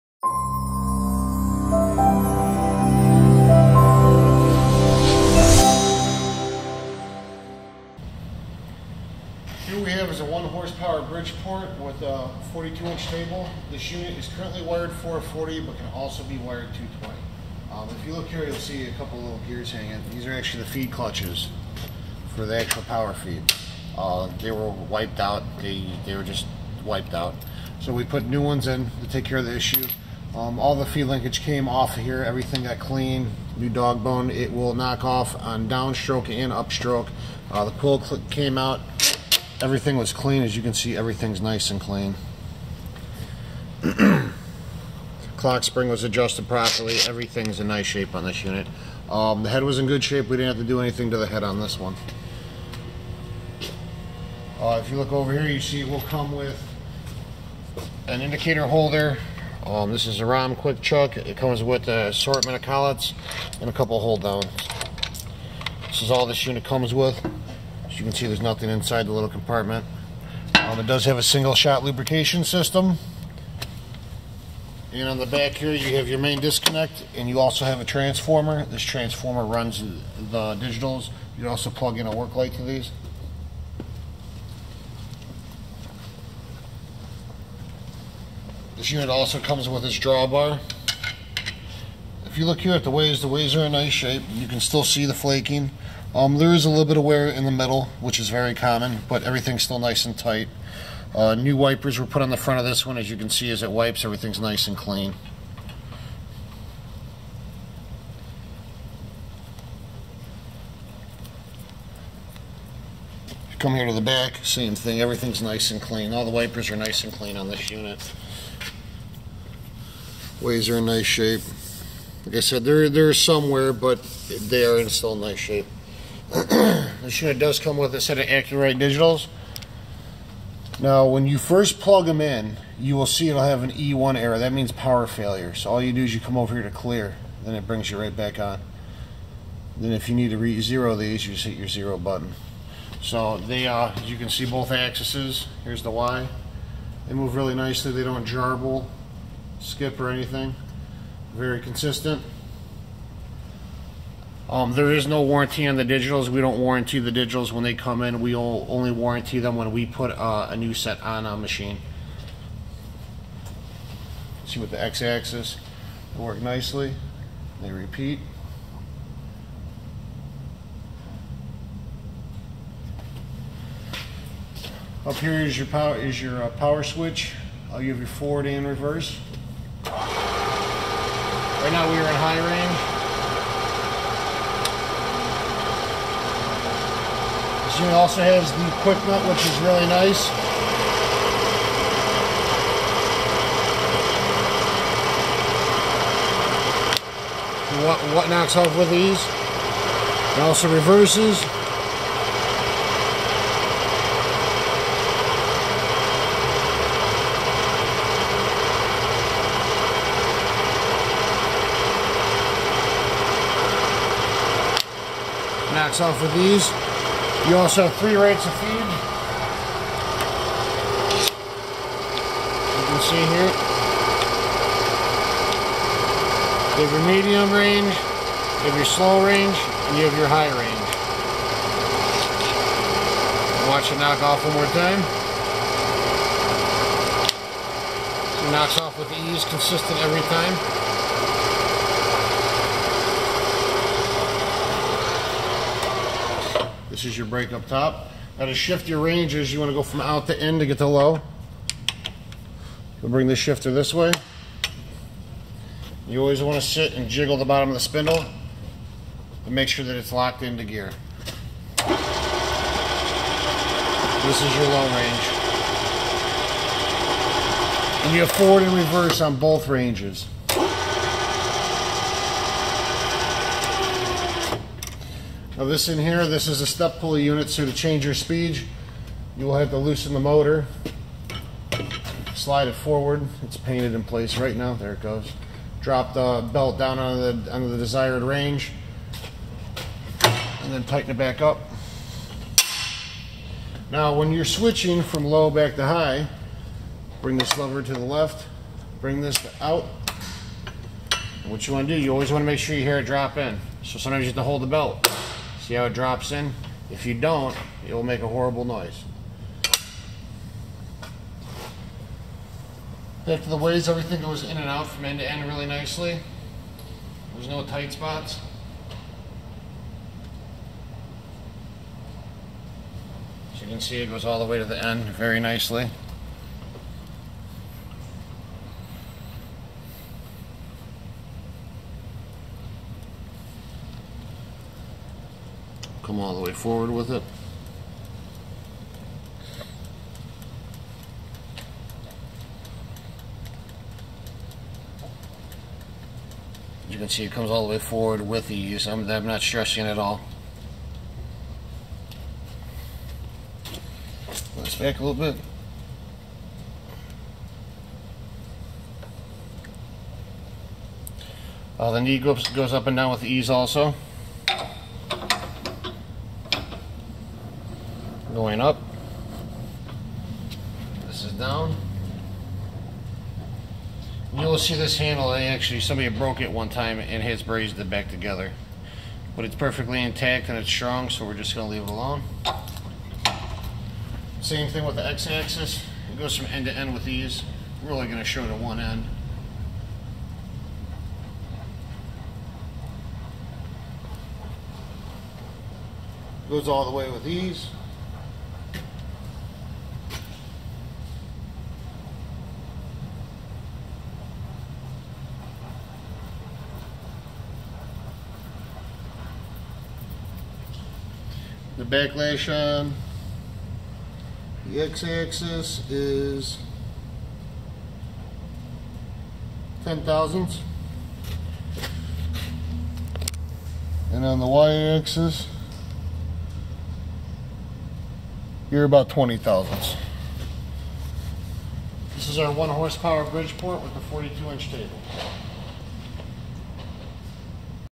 Bridgeport 1 HP 9" x 42" Vertical Milling Machine
BRIDGEPORT 1 HP 9” x 42” Vertical Mill
View eBay Listing

Small Tools Inventory #: S21747

Brand: Bridgeport
Type: Vertical Mill
Style: Step Pulley Head
Serial #: 100483
8 Spindle Speeds: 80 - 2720 RPM
Spindle Taper: R8
Spindle Travel: 5”
Three (3) settings for spindle feed
Table size: 9” x 42”
2-axis Act-Rite digital readout
R8 collet rack with collets
Rohm keyless drill chuck with 3/8” capacity

HP: 1
Volts: 440
Phase: 3
Cycle: 60

Small Tools Inc.
26470 Lakeland Blvd.

We buy and sell new, used, and reconditioned machinery and tooling.